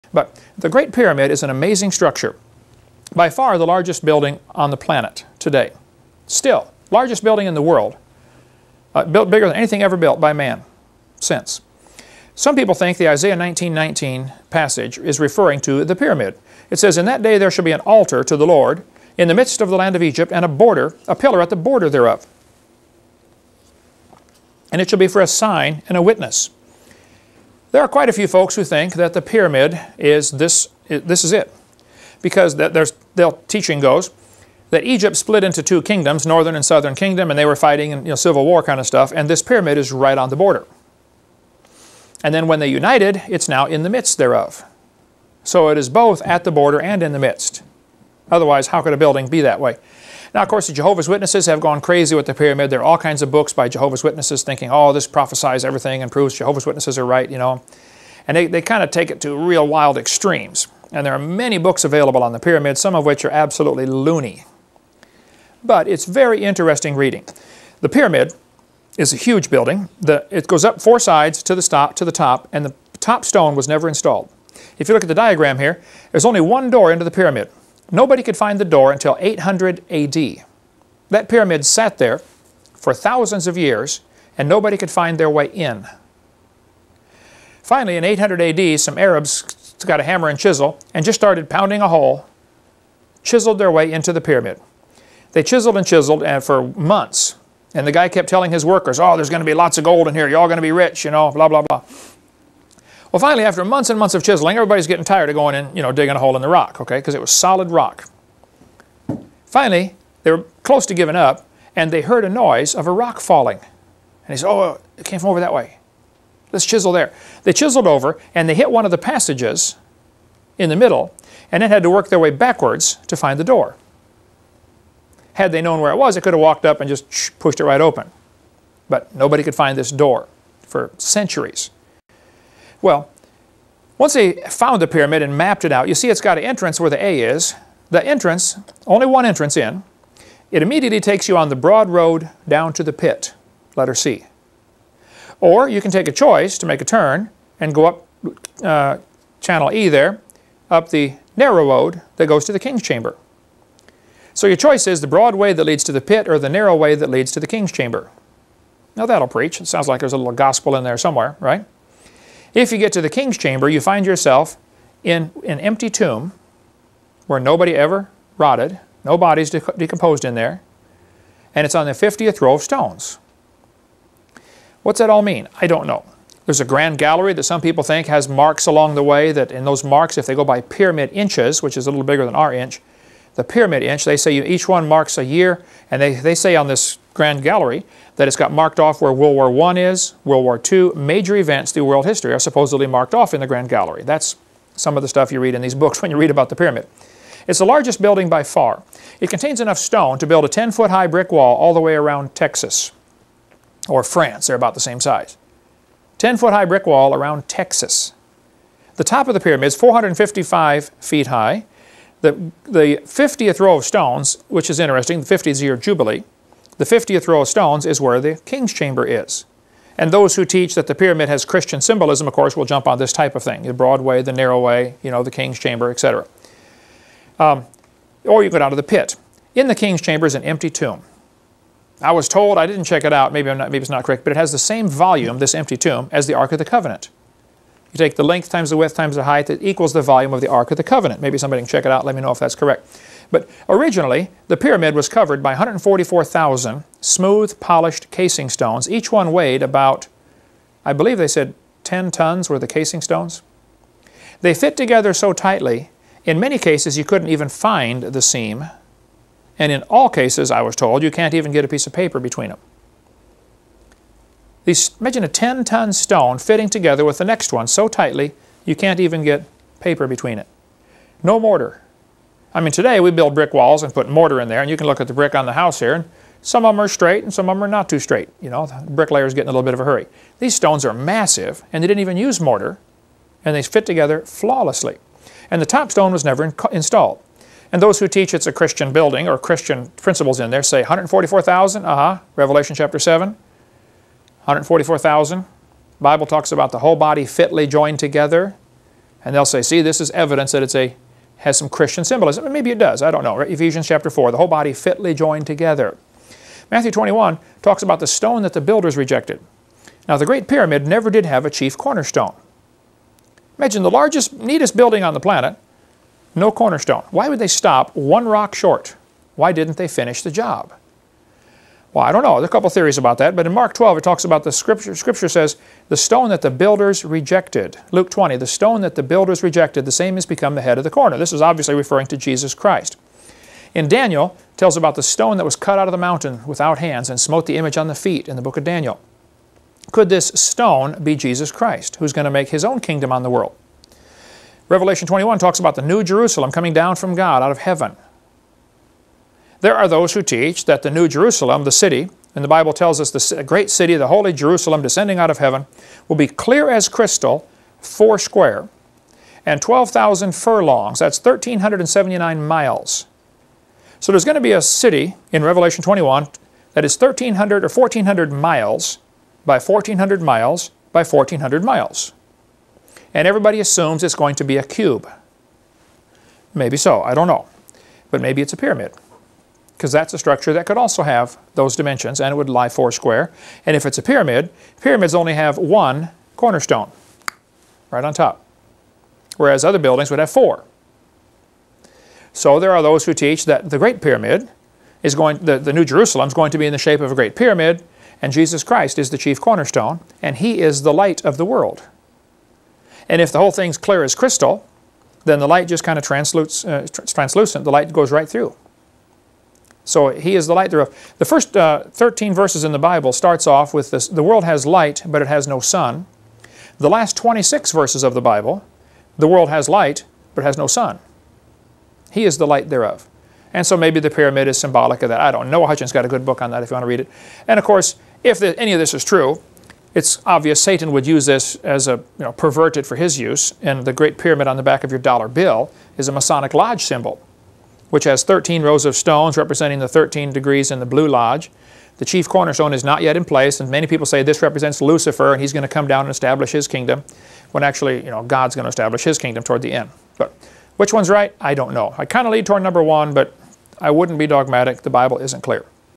The great pyramid - Kent Hovind
This clip was taken from seminar 7 (Questions and answers)